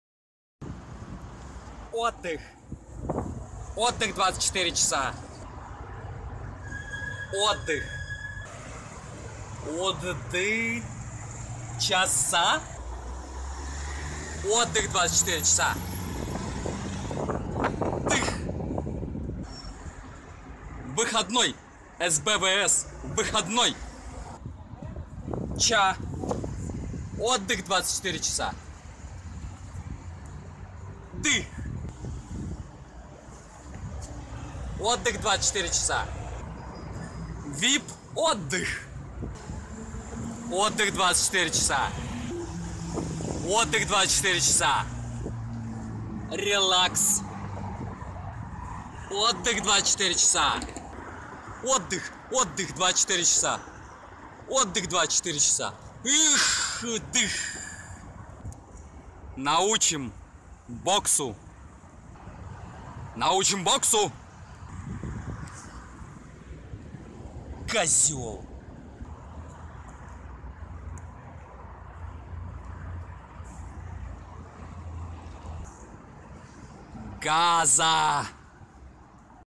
"ОТДЫХ 24 часа"
В данном ролике поэзия возникает не из идей и даже не из дискурсивных практик, а из повседневных фрагментов и буквальных обрывков языка, потерпевшего катастрофу в пучине уличной рекламы круглосуточного отдыха. Автор видеоряда приходит всегда слишком поздно, когда язык руинирован и остались только его осколки, разбросанные тут и там анонимными распространителями бесконечного круголосуточного Отдыха. Видеоряд следует по следам этого безавторского городского письма и дополняется аффективными вскриками - всполохами релаксационной обсессии, дополненными полной беспомощностью любых интерпретаций при прочтении. Поэтическая речь, которая еще в эпоху романтизма была примером речи, освобожденной от бремени повседневной необходимости, заново пересобирает себя из этих обрывков, превращая языковую обсессию в суггестивную ткань новой поэзии, пришедшей на руины языка как поэзия общества после работы. Эта новая поэзия обещает долгожданный отдых самому слову "Отдых", до сих пор вынужденному трудится 24 часа в сутки.